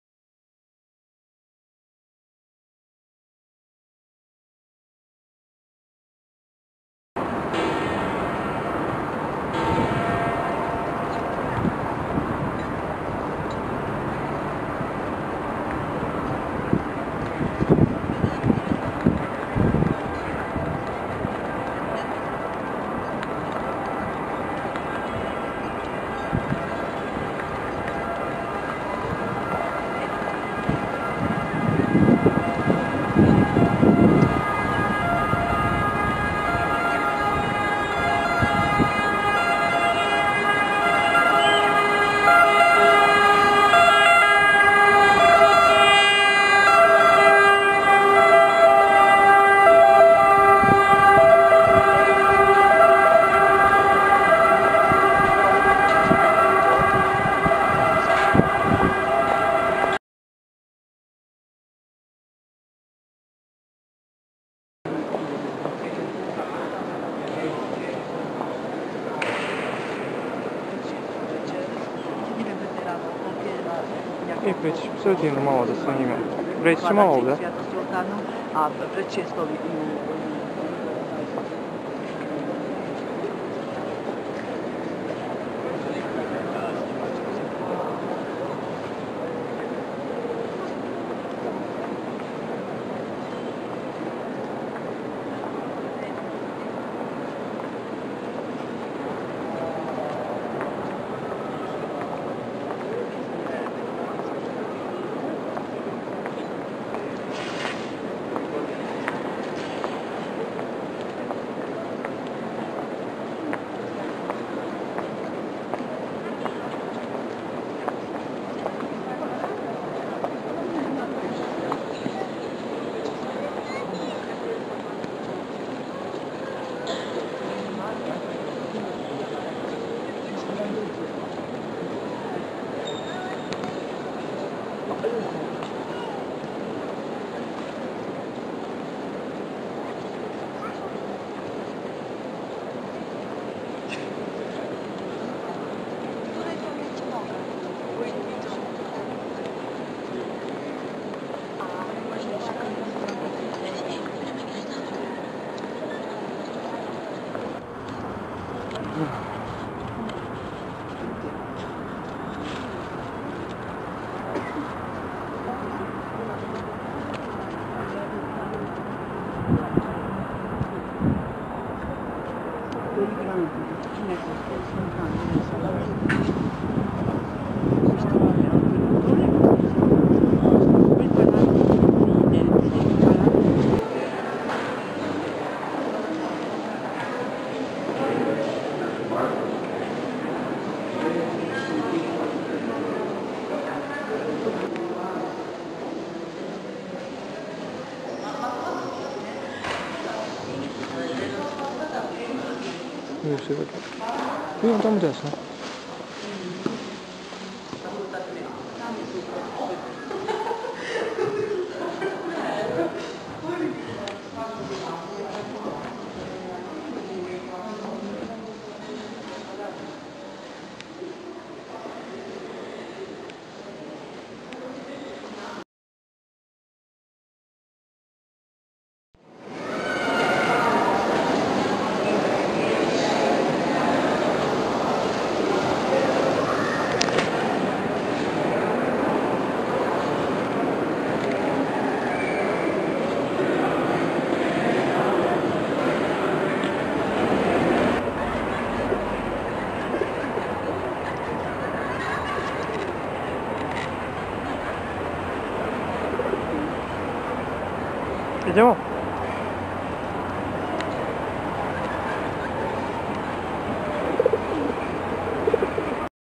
St. Peter Basilica - Rome
Inside Saint Peter's Basilica, Vatican City, Rome, Italy
St. Peter's Basilica is a Late Renaissance church located within Vatican City.

Memories from trip to Rome, march 2010,

Movie: Bojan Pavlukovic